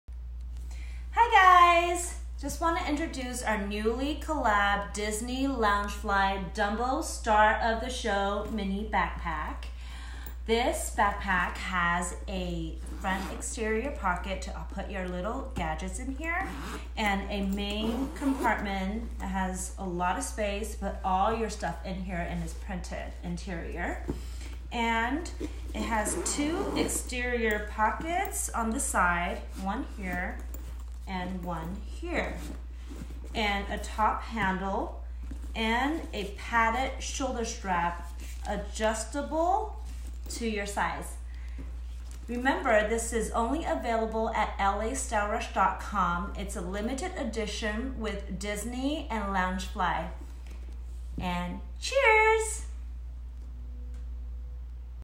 Loungefly X LASR Exclusives Disney Dumbo Star of the Show Mini Backpack
The Loungefly X LASR Disney Dumbo Star of the Show Mini Backpack is only exclusive to our site. Grab yours here:

Roll up roll up the circus is coming! Make your looks and spirits soar with the Loungefly X LASR EXCLUSIVE Disney Dumbo Stripe Star of The Show Mini Backpack. Marvel at this wonderful mini festival backpack featuring an exquisite powder blue exterior, delightful contrast circus stripe front pattern, adorable debased graphic of Dumbo, the magnificent flying elephant and dazzling gold toned hardware. Includes red lining, a large main compartment, zippered exterior front pocket, adjustable shoulder straps and top zip closure, it’ll be no miracle that you look super cute wearing this magical Disney backpack.

* Width: 9" x Height: 12.5" x Depth: 4.5"
* Faux leather
* Striped / Dumbo details
* Top zipper closure
* Fully lined interior
* Logo metal hardware
* Front zippered pocket
* Adjustable straps
* Top handle